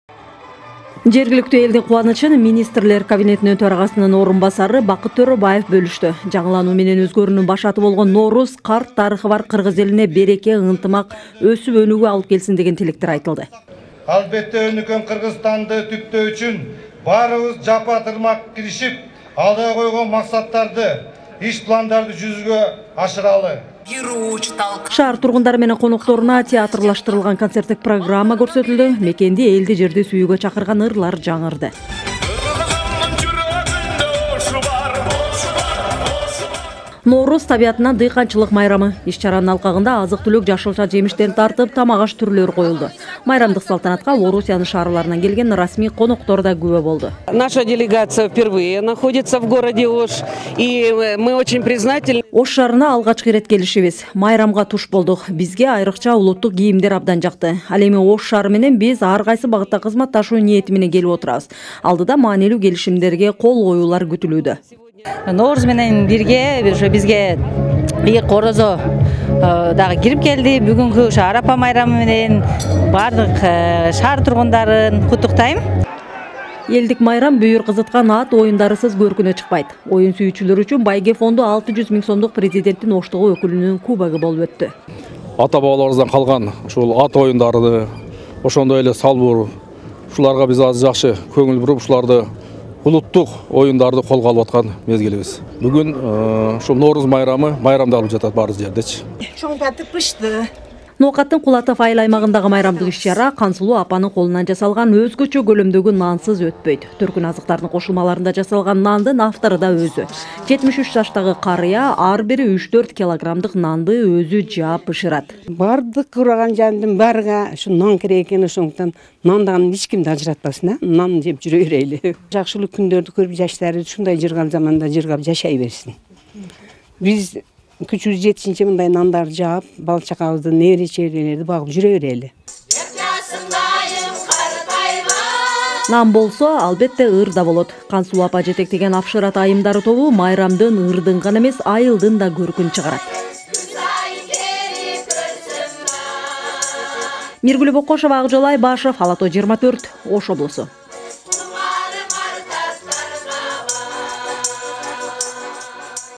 Ош: Нооруз - Көктөм майрамы!
Ош жергесинде өткөн көктөм майрамына Министрлер Кабинетинин Төрагасынын орун басары Бакыт Төрөбаев катышты. Көп улуттуу эл жашаган калаада элдик майрам өзгөчө шаң менен белгиленди.